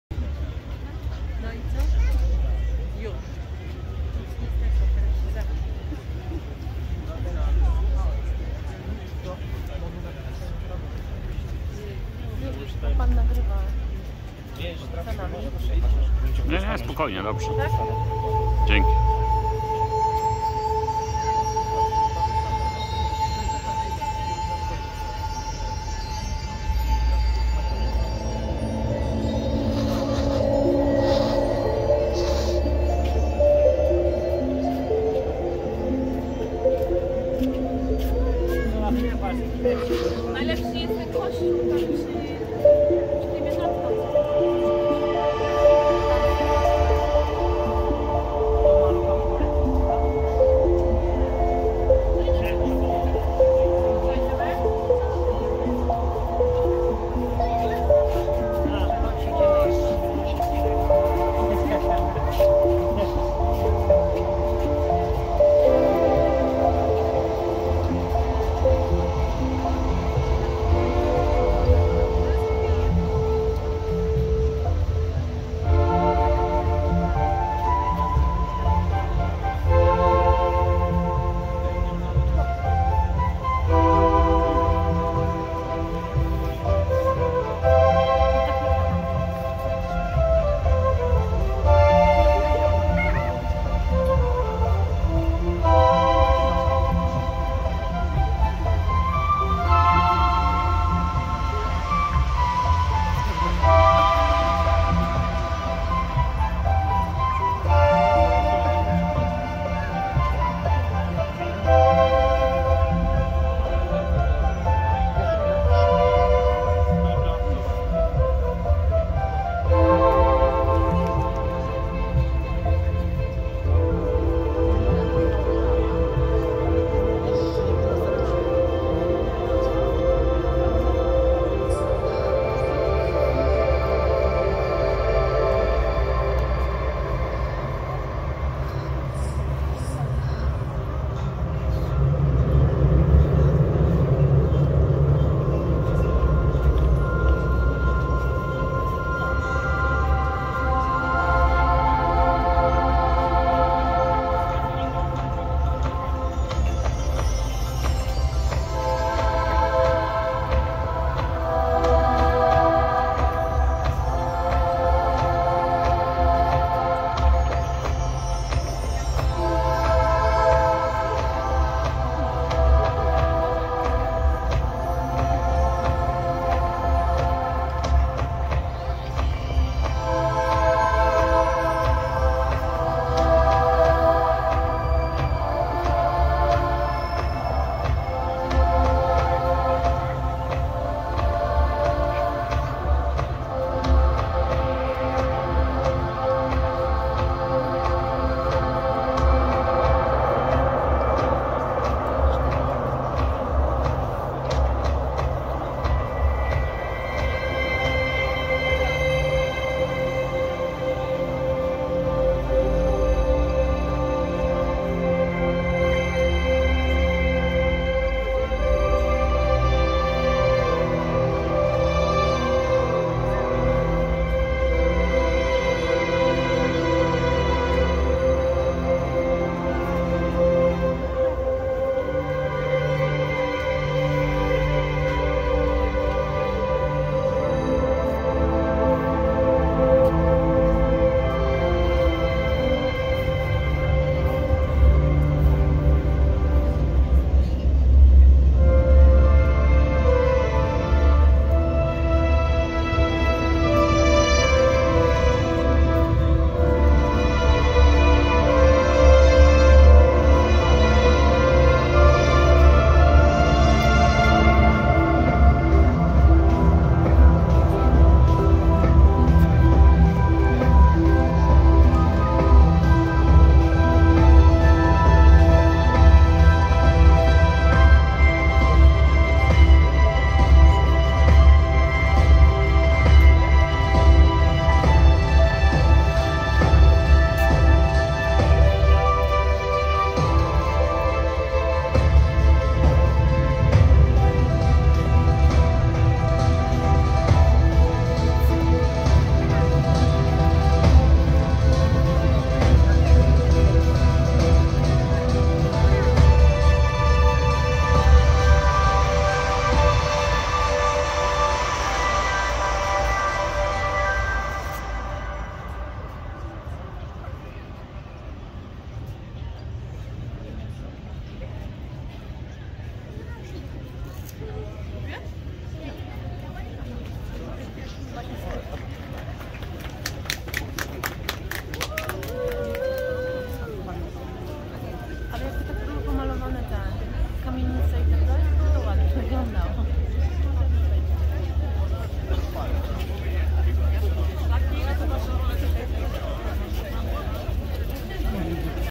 Light Move Festiwal 2022: Mapping "Breath" Cerkiew na ul. Kilińskiego
Light Move Festiwal 2022 w Łodzi. Mapping "Breath" na Soborze Św. Aleksandra Newskiego w Łodzi. Autor: Leslie Epsztein, Camille Gross (Cerkiew - Jana Kilińskiego 56) - Festiwal Kinetycznej Sztuki Światła, Light Move Festival 2022 w Łodzi.